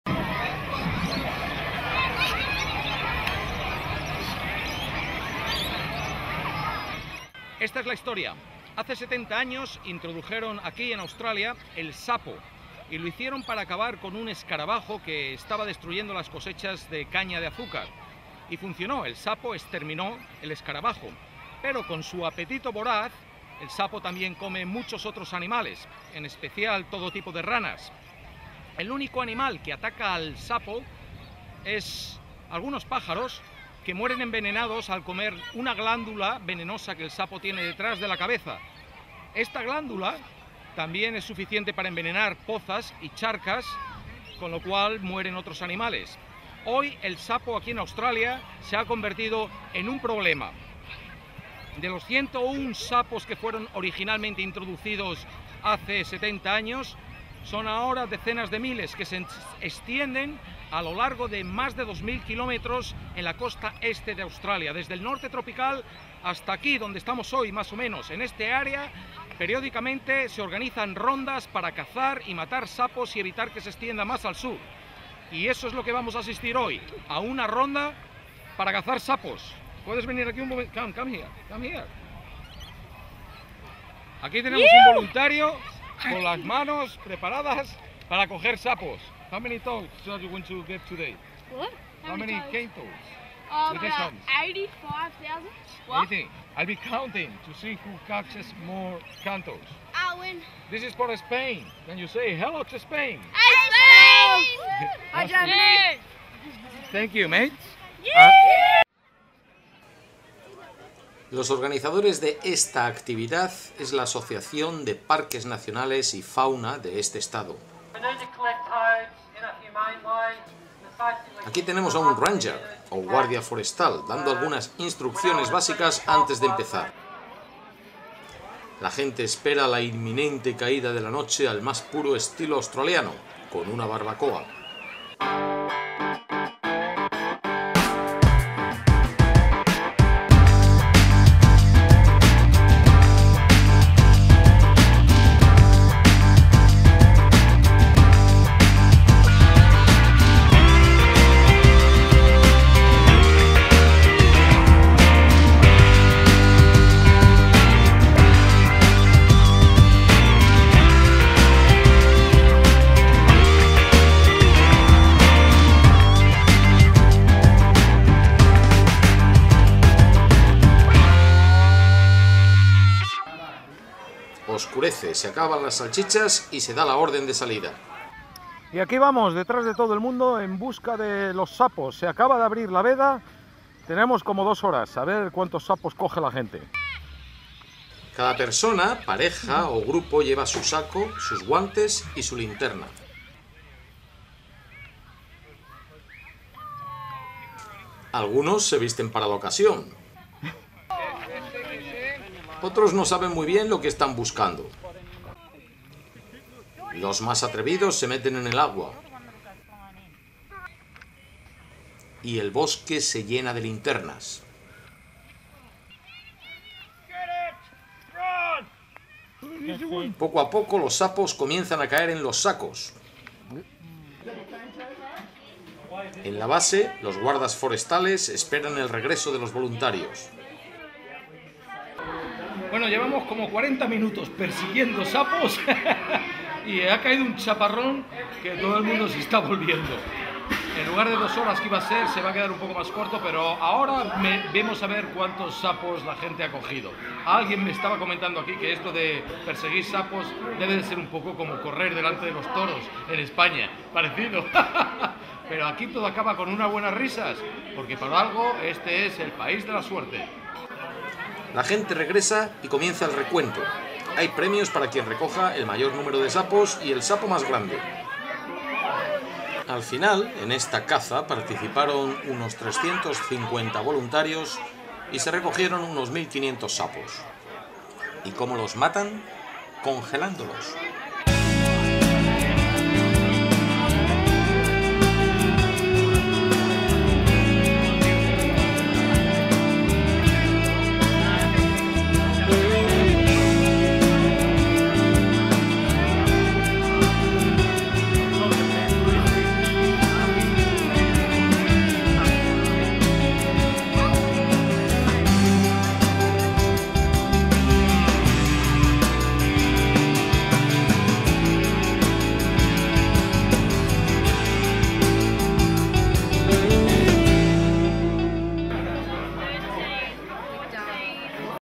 Caza del Sapo en Australia
Una manera australiana de pasar la tarde de un domingo